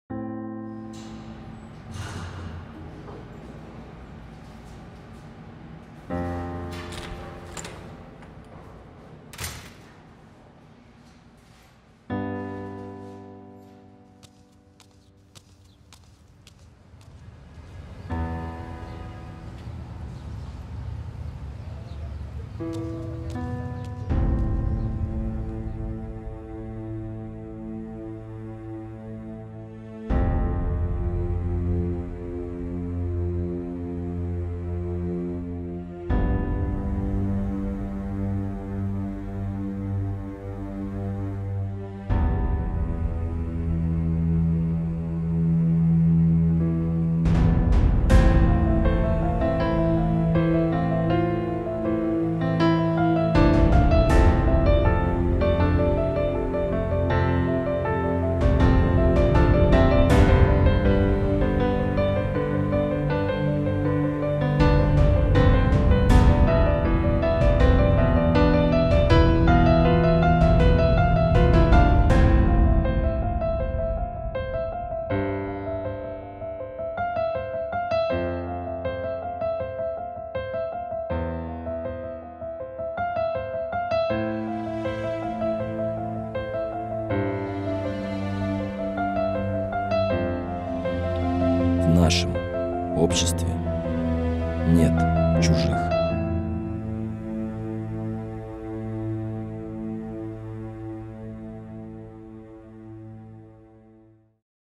социальный ролик "Мир инвалидов" (petrucho studio)
Социальный ролик в поддержку людей с ограниченными возможностями. В рамках проекта "Доступная среда".